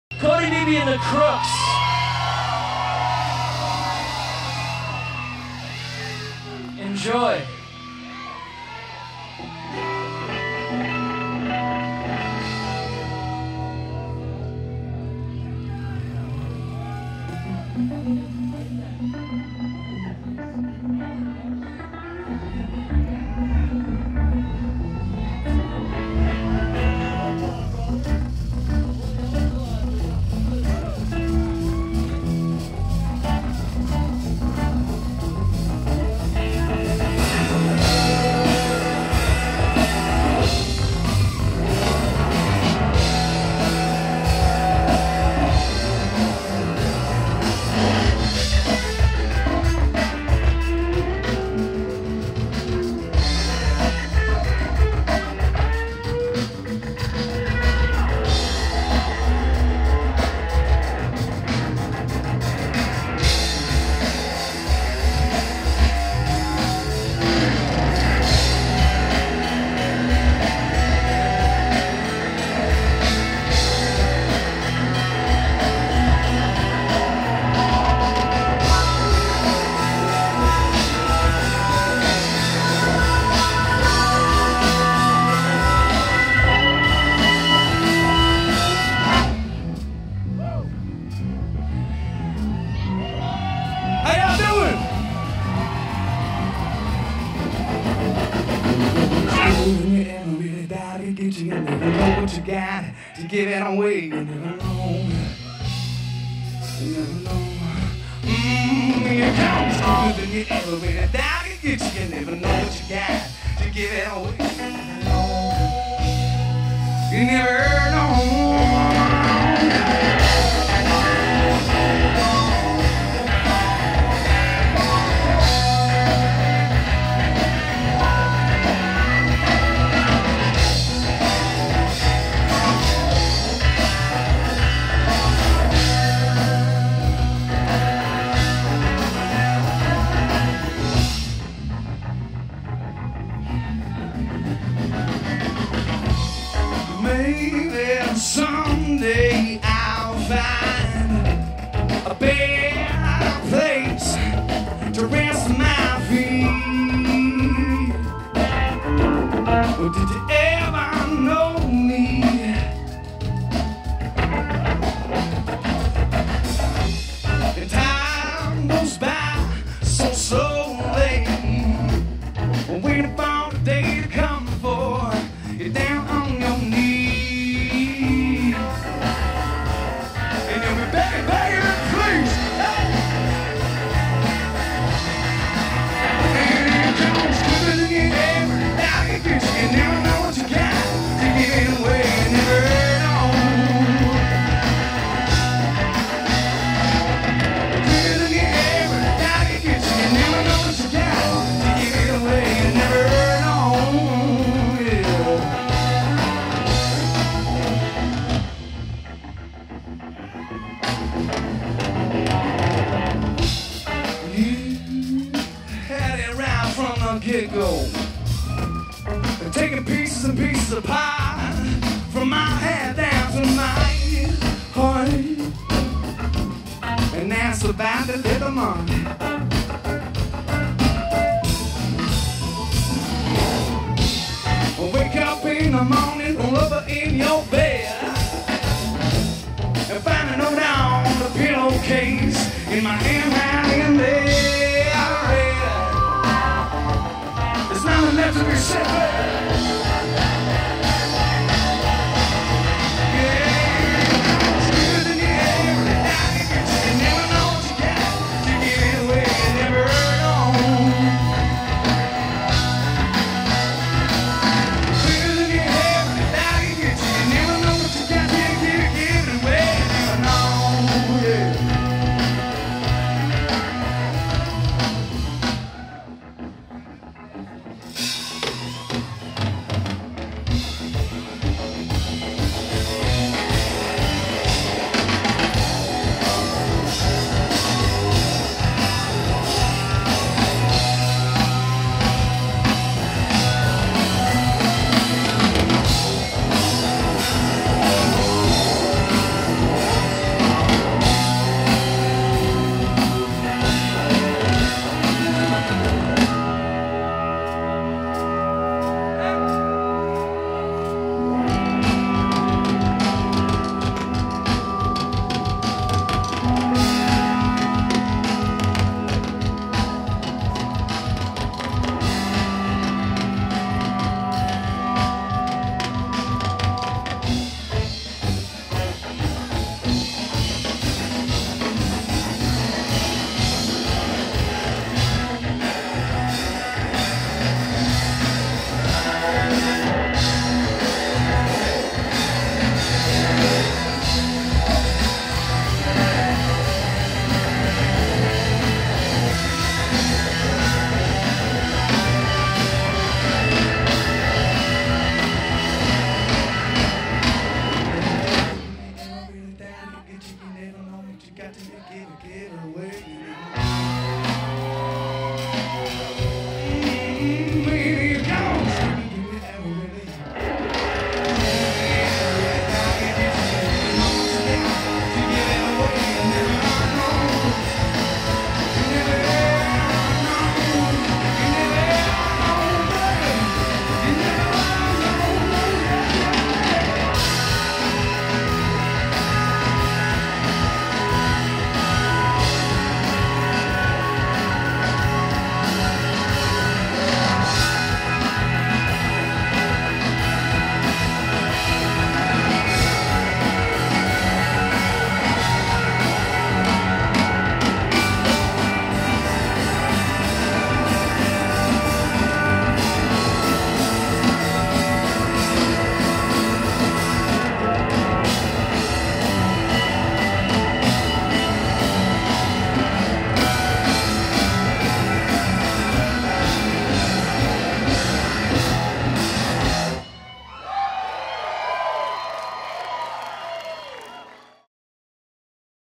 Cody Beebe & The Crooks Live at Chinook Music Festival
Festival founders Cody Beebe & The Crooks performed Saturday night on the Ted Brown Music Main Stage.